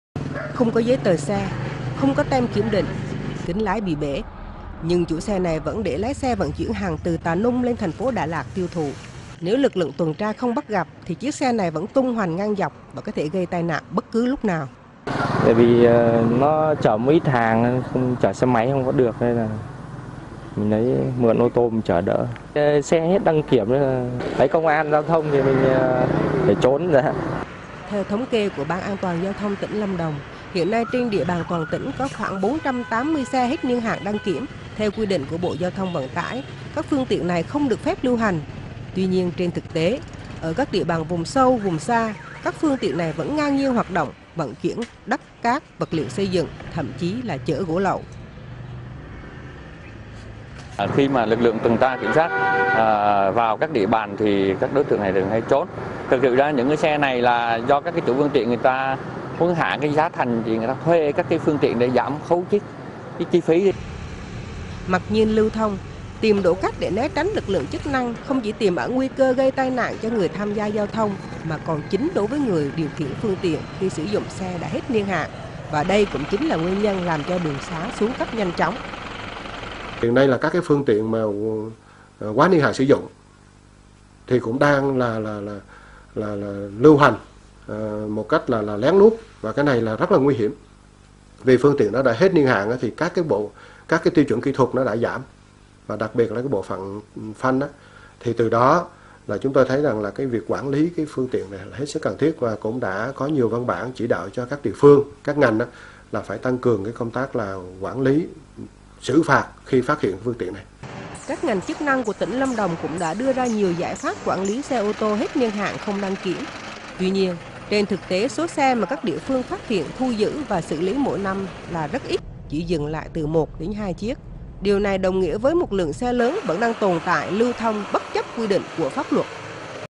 Lâm Đồng: Xe hết niên hạn vẫn tung hoành ngang dọc
Không chỉ đe dọa tính mạng con người, những chiếc xe hết niên hạn còn làm cho các tuyến đường giao thông xuống cấp trầm trọng vì quá khổ, quá tải.